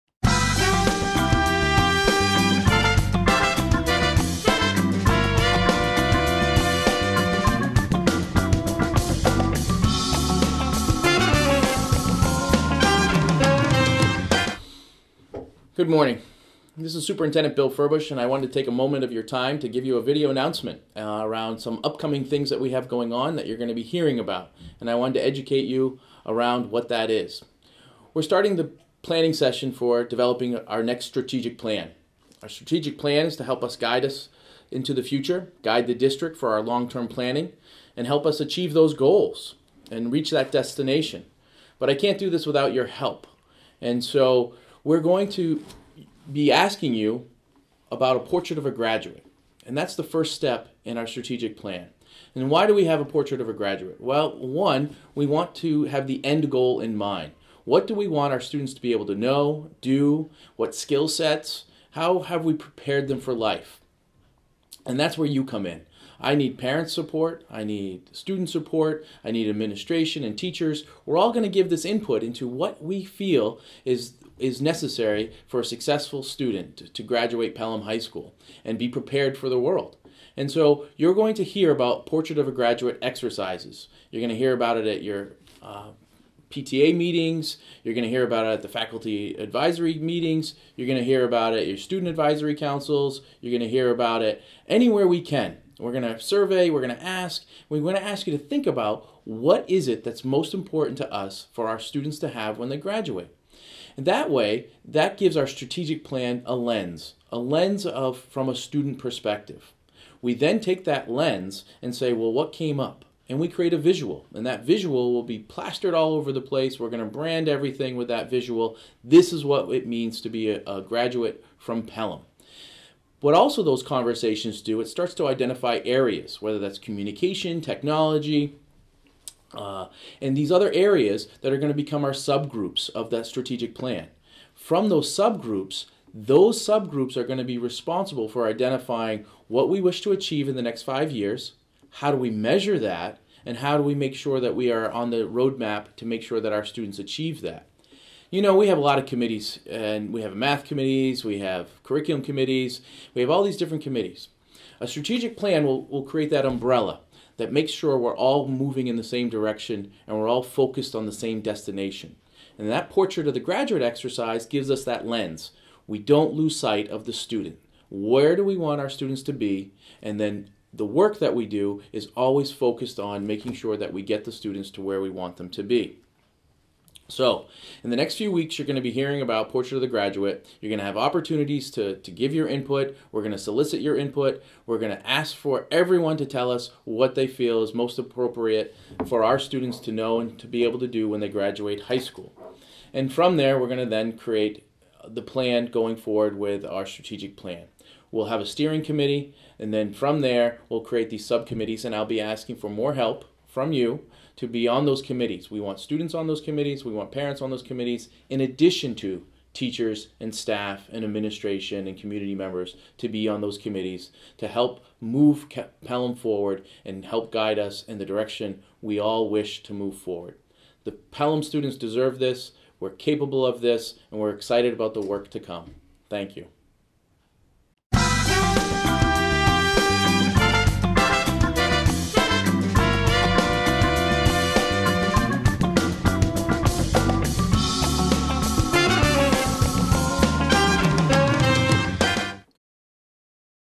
Pelham School District "Portrait of a Graduate"
This is a short video announcement from Superintendent Furbush explaining the purpose of the Portrait of a Graduate exercise and how this exercise will be used as the first phase of the Strategic Planning process. We are looking to involve as many stakeholders in this exercise as possible and create a student lens in order to view and create the next Strategic Plan in Pelham, New Hampshire.